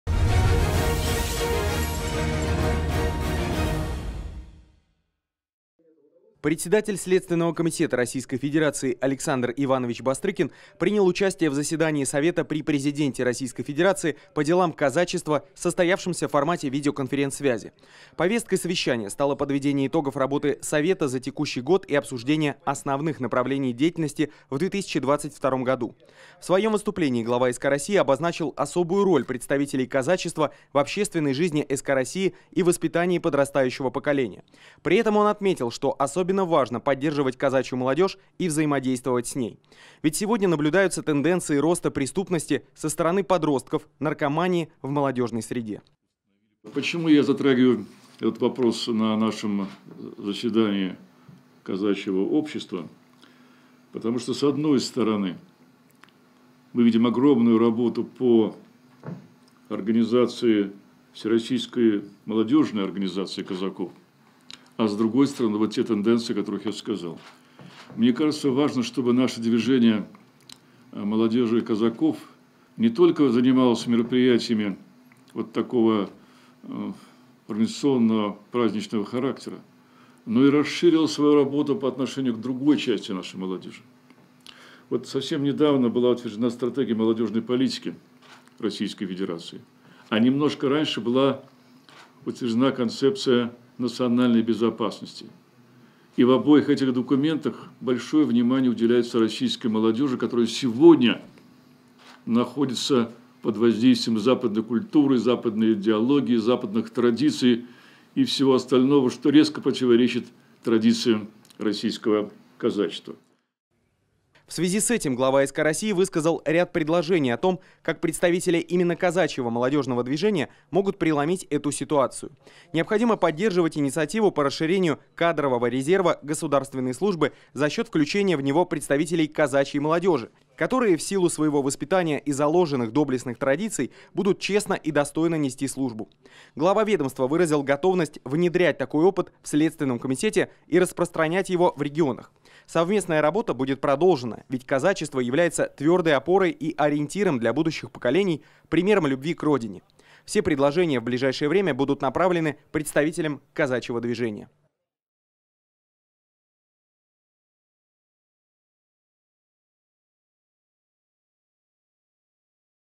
Председатель СК России принял участие в заседании Совета при Президенте РФ по делам казачества
Председатель Следственного комитета Российской Федерации Александр Иванович Бастрыкин выступил на заседании Совета при Президенте Российской Федерации по делам казачества, состоявшемся в формате видео-конференц-связи. В нем  приняли участие представители Министерства обороны, Министерства внутренних дел,   Министерства просвещения, Министерства природных ресурсов и экологии РФ;  Федерального агентства по делам национальностей; Комитета Государственной Думы по делам Содружества Независимых Государств, евразийской интеграции и связям с соотечественниками.

Повесткой совещания  стало подведение итогов работы совета за текущий год и обсуждение основных направлений деятельности в 2022 году.

В своем выступлении глава СК России обозначил особую роль представителей казачества в воспитании подрастающего поколения. Говоря о росте преступности и наркомании в подростковой среде, он  отметил, что особенно важно поддерживать представителей молодежного казачьего движения, которые являются примером и верным нравственным ориентиром. В частности, А.И. Бастрыкин отметил необходимость поддержать инициативу по расширению кадрового резерва государственной службы за счет включения в него представителей казачьей молодежи, которые в силу воспитания и заложенных доблестных традиций будут честно и достойно выполнять свои обязанности. Он выразил готовность внедрять такой опыт в Следственном комитете и распространять его в регионах. Также А.И. Бастрыкин предложил молодежному казачьему сообществу подумать над разработкой медиасети, чтобы использовать современные цифровые технологии в позитивном ключе для создания положительных программ в сети Интернет и более дружественного к детям медиапространства.

«Почему я затрагиваю этот вопрос на нашем заседании Казачьего общества? Потому, что, с одной стороны, мы видим огромную работу по организации Всероссийской молодежной организации казаков, а с другой стороны - наблюдаем те тенденции, о которых я сказал. Мне кажется важным, чтобы  движение казаков расширило свою работу по отношению к другой части нашей молодежи», - добавил А.И. Бастрыкин.

На совещании также прозвучали доклады, посвященные  вопросам совершенствования законодательства Российской Федерации в сфере реализации государственной политики в отношении казачества. Были подведены итоги конкурса «Лучший казачий кадетский корпус». Обсуждалась тема привлечения российского казачества к участию в обеспечении природоохранной деятельности, экологической безопасности и охране лесов. Поднят вопрос  о  развития международных контактов с казаками-соотечественниками, проживающими за рубежом.

Совместная работа будет продолжена, ведь казачество – это опора государства Российского. Все предложения в ближайшее время будут направлены представителям казачьего движения.